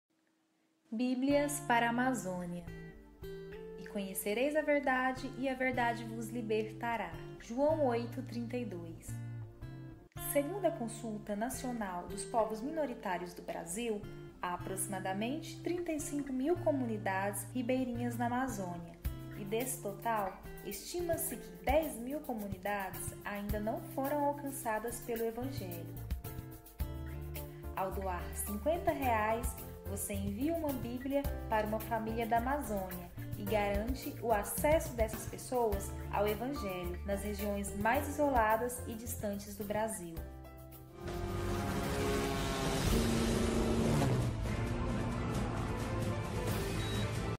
Bíblias para Amazônia
"E conhecereis a verdade, e a verdade vos libertará." João 8:32.

Segundo a Consulta Nacional dos Povos Minoritários do Brasil, há aproximadamente 35 mil comunidades ribeirinhas na Amazônia e desse total, estima-se que 10 mil comunidades ainda não foram alcançadas pelo Evangelho.

Ao doar R$ 50,00 (cinquenta reais) você envia uma Bíblia para uma família da Amazônia e garante o acesso dessas pessoas ao Evangelho nas regiões mais isoladas e distantes do Brasil.